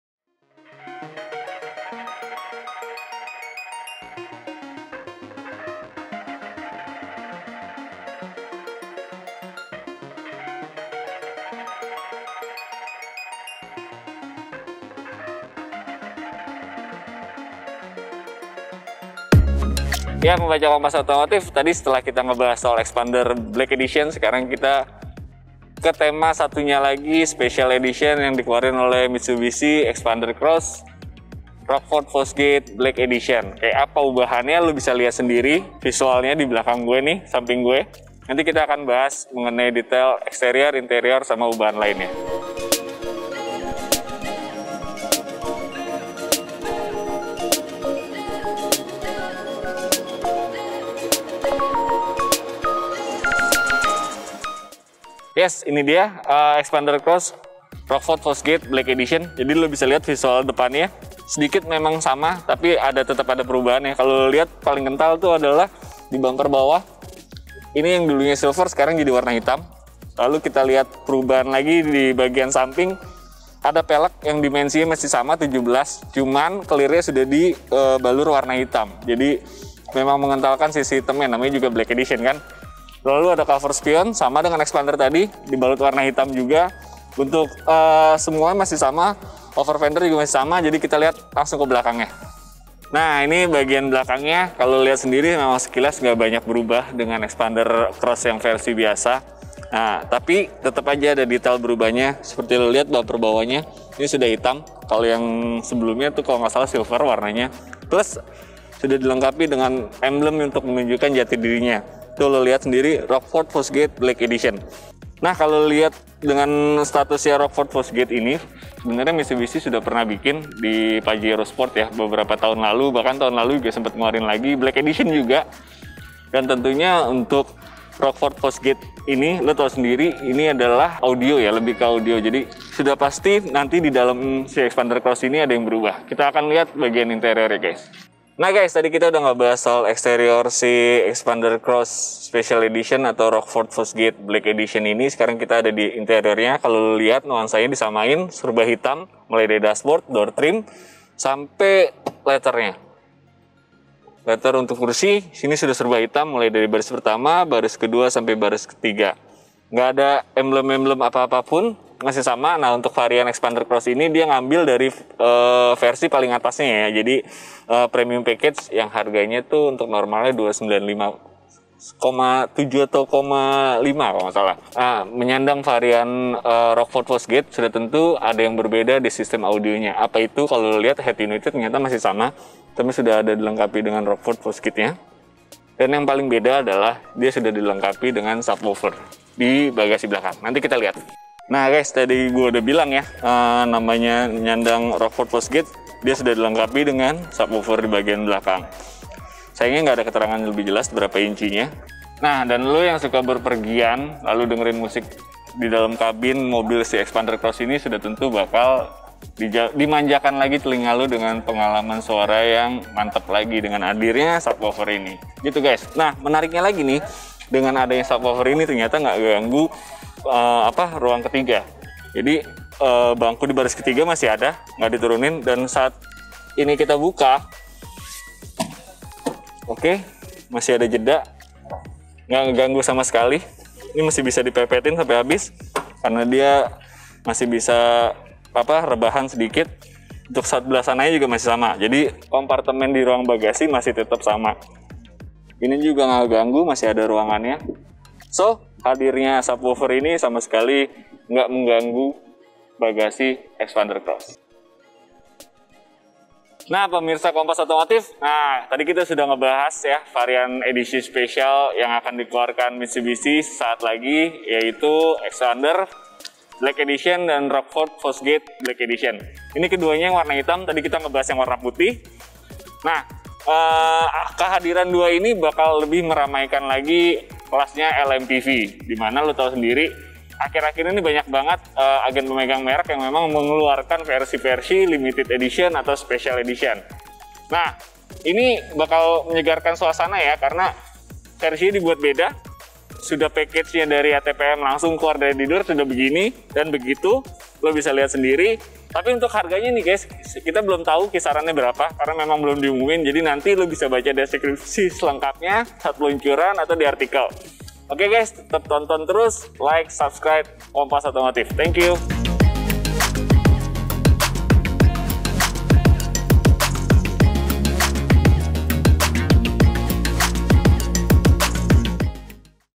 Mitsubishi Xpander Cross Rockford Fosgate Black Edition Rp 300 Juta-an
Mitsubishi menghadirkan dua penyegaran pada dua model terpopulernya yakni Xpander dan Xpander Cross. Khusus untuk MPV ala SUV Xpander Cross, perubahan dibuat mengikuti saudaranya Pajero Sport yang beberapa waktu lalu dihadirkan juga edisi Rockford Fosgate.

Rockford Fosgate merupakan merek yang dikenal dengan produk audionya. Xpander Cross diberikan peningkatan pada bagian audio untuk meningkatkan pengalaman mendengarkan hiburan yang lebih baik. Sesuatu yang cocok untuk penggemar audio atau biasa disebut audiofil.

Banderol harganya Rp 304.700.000 (OTR DKI Jakarta).

Seperti apa ubahannya? Simak video berikut

Laporan ini disampaikan oleh
Reporter : Stanly Ravel
Video Editor :Setyo Adi

Selamat menonton

Jika Anda menyukai video ini, tekan tanda jempol ke atas.

Kompas.com